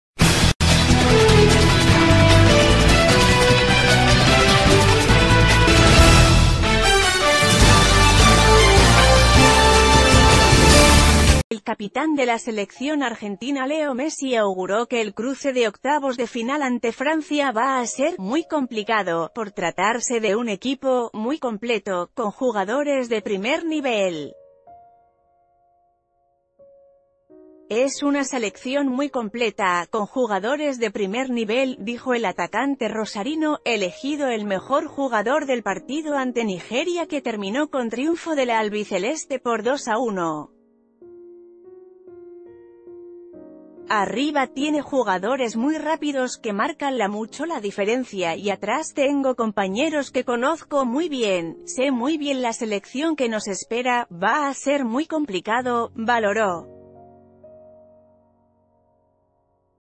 Messi: Francia es una selección muy completa
El capitán de la selección argentina Leo Messi auguró que el cruce de octavos de final ante Francia va a ser "muy complicado" por tratarse de un equipo "muy completo, con jugadores de primer nivel". "Es una selección muy completa, con jugadores de primer nivel", dijo el atacante rosarino, elegido el...